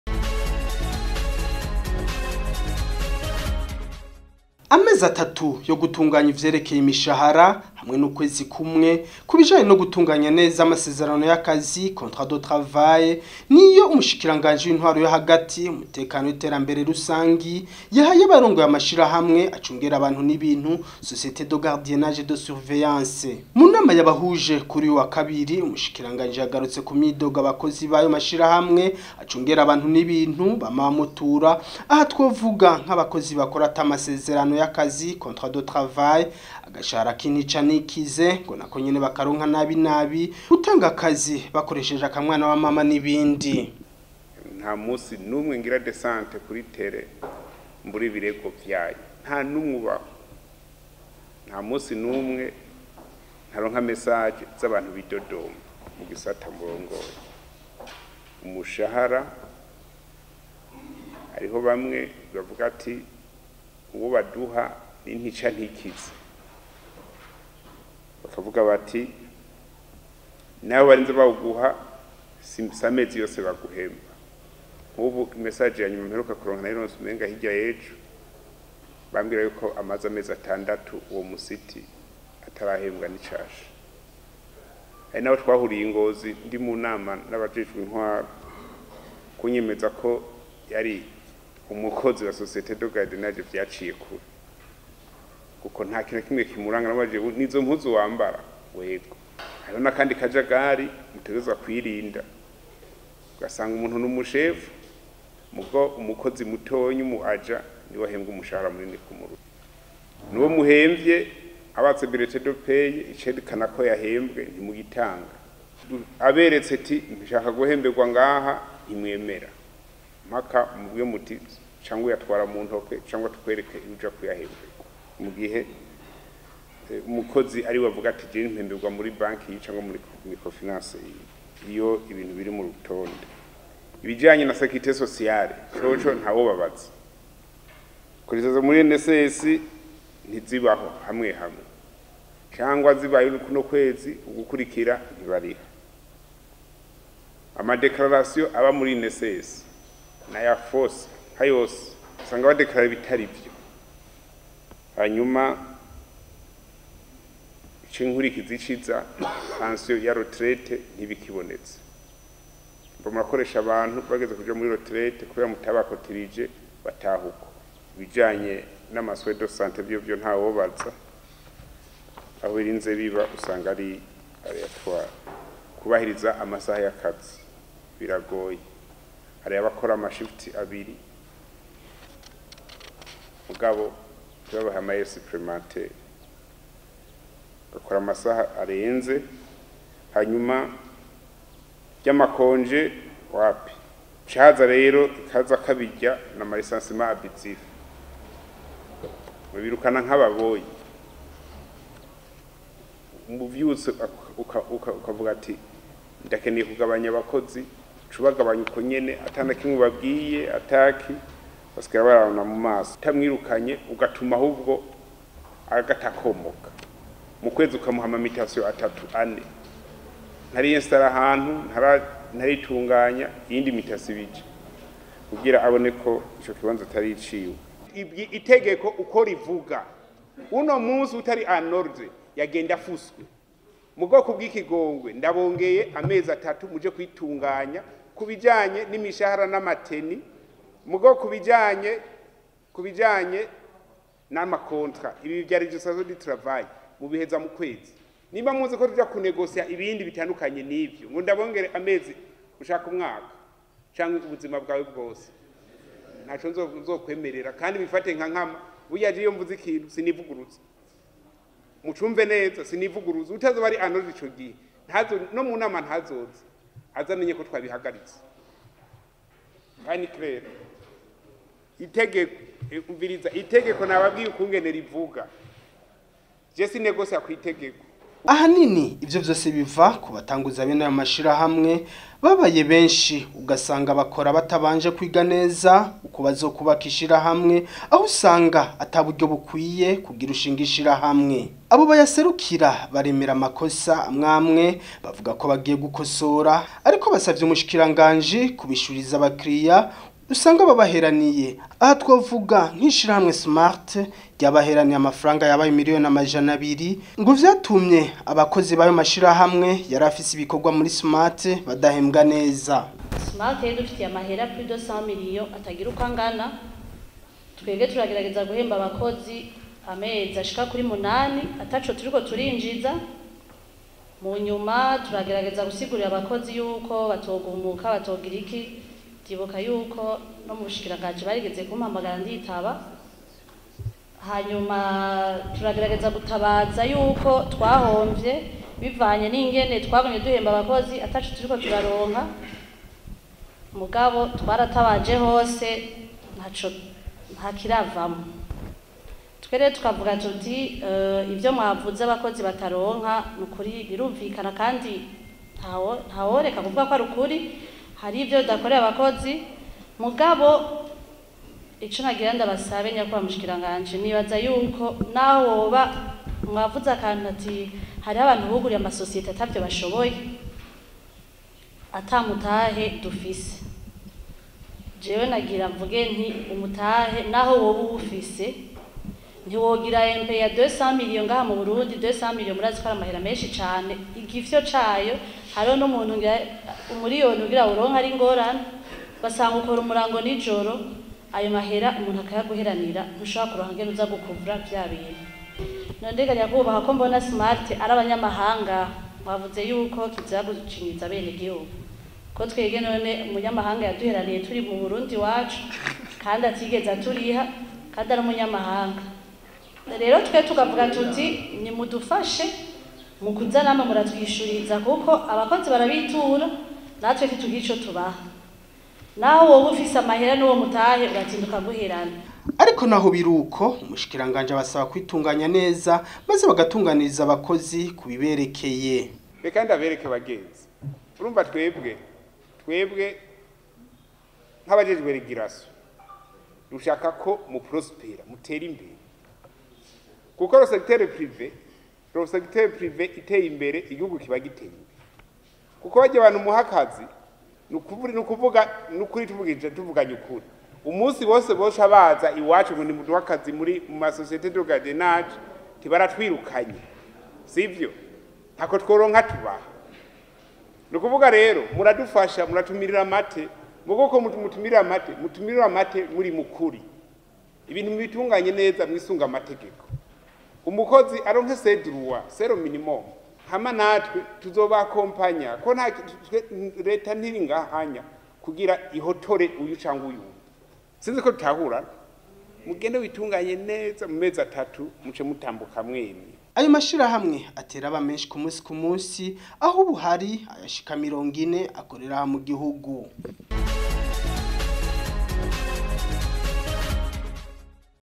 Umushikiranganji w'Intwaro yo hagati aragabisha abajejwe amashirahamwe ajejwe gucungera abantu.
Burundi Amezi atatu gusa kugira babe batunganije ivy'imishahara, ukwezi kumwe gusa kugira babe batunganije n'amasezerano y'akazi niyo Umushikiranganji w'Intwaro yo hagati mu gihugu, iterambere rusangi n'umutekano yahaye abarongoye amashirahamwe ajejwe gucungera abantu n'ibintu mu nama yabahuje.